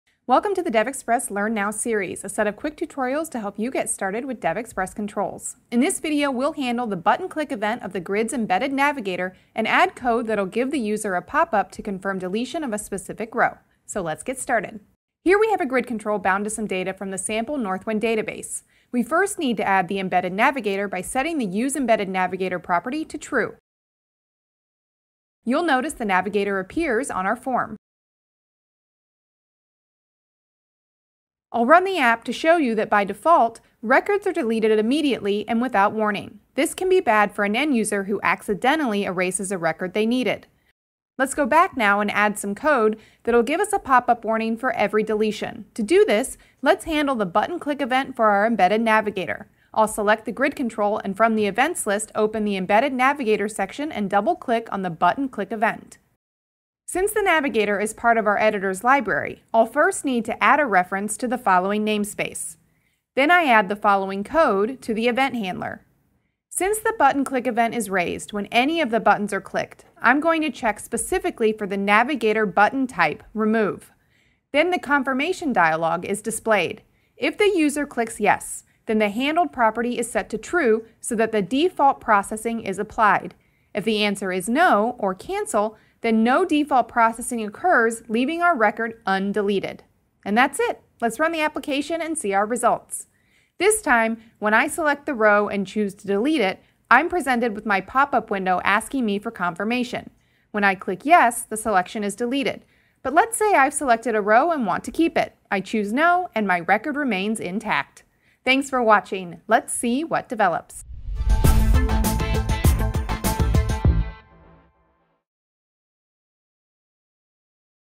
WinForms Data Grid: How to Display a Popup Warning when Deleting a Row
In this video you'll learn how to display a popup warning for end-users that delete records.